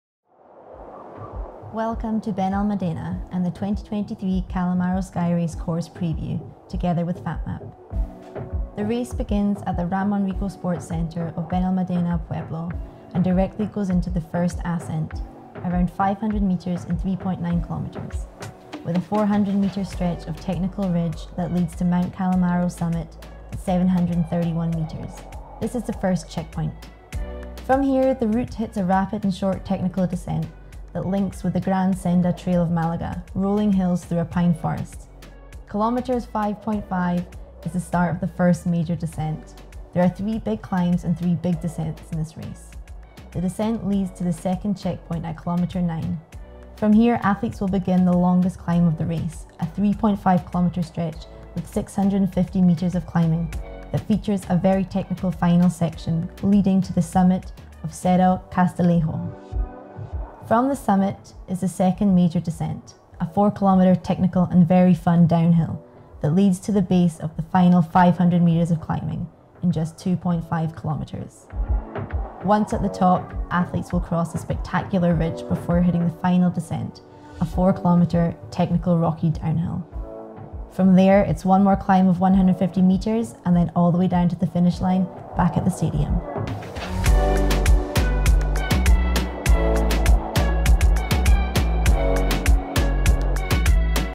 CALAMORRO SKYRACE: COURSE PREVIEW
Check out the course profile of the first race of the 2023 Skyrunner World Series, Calamorro Skyrace on April 29th 2023.
Mapping provided via the SWS official mapping partner - FATMAP.

2023 Skyrunner® World Series